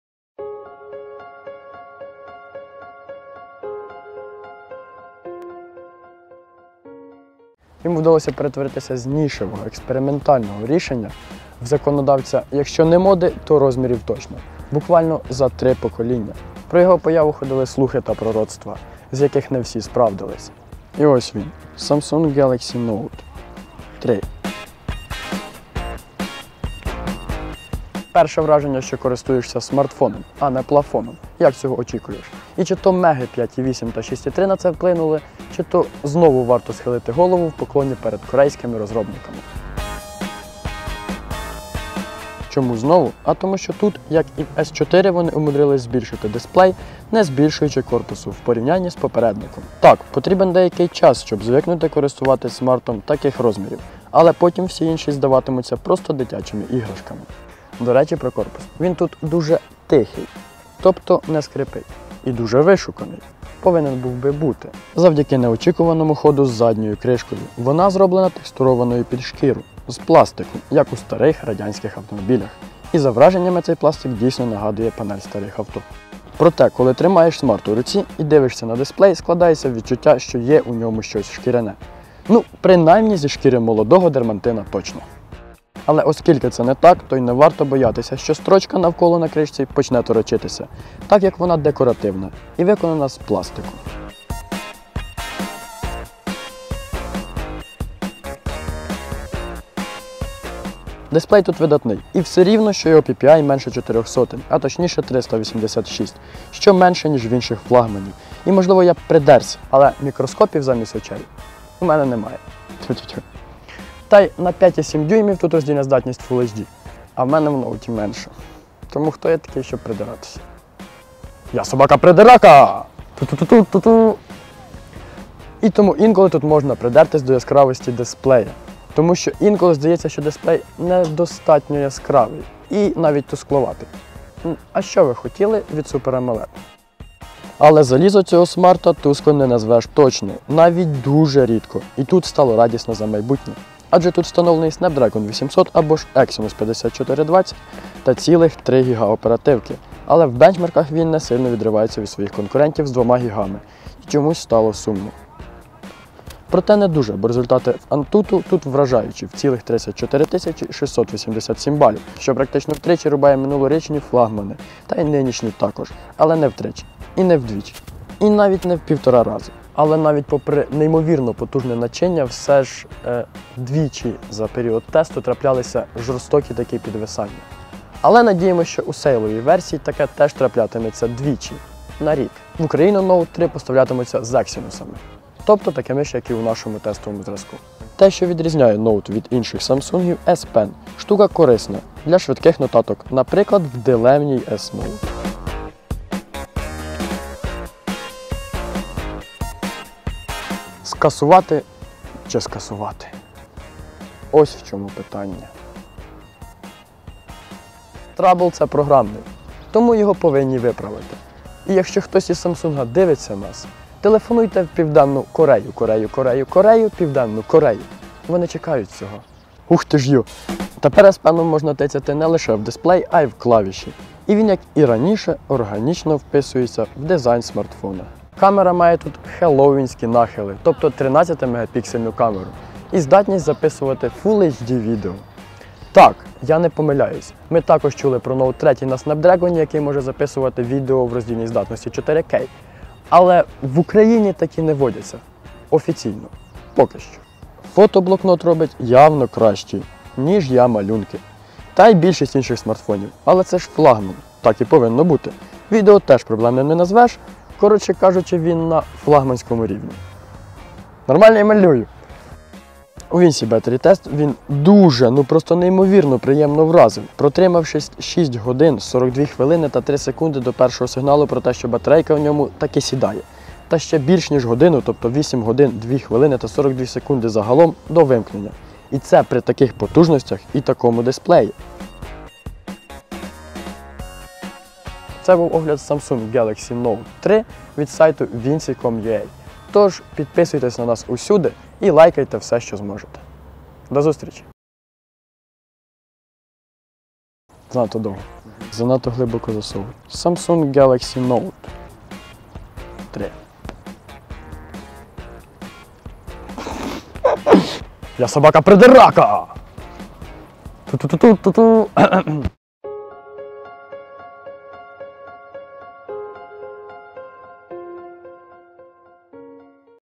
Samsung Galaxy Note 3
Йому вдалось перетворитись з нішевого експериментального рішення в законодавця, якщо не моди, то розмірів точно, всього лиш за три покоління. Щодо його появи ходили легенди і пророцтва, з яких не всі підтвердились, і ось він Samsung Galaxy Note 3 -- третій з перших.